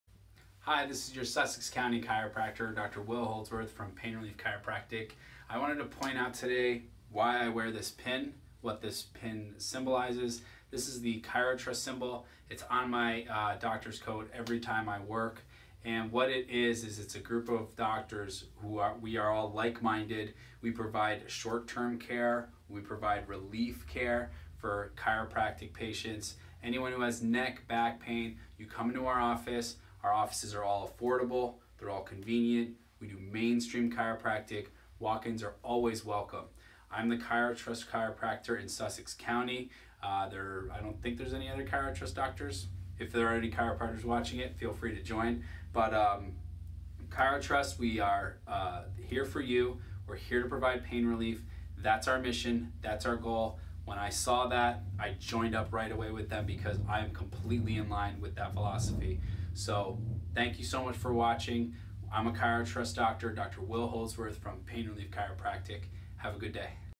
What's With That Pin You Wear On Your Doctor's Coat?
Newton Andover NJ chiropractor Dr. William Holdsworth DC from Pain Relief Chiropractic LLC explains why he wears a pin on his doctor's coat and what it symbolizes.  Chiro-Trust is a group of like minded doctors providing short-term mainstream chiropractic care in a convenient and affordable way.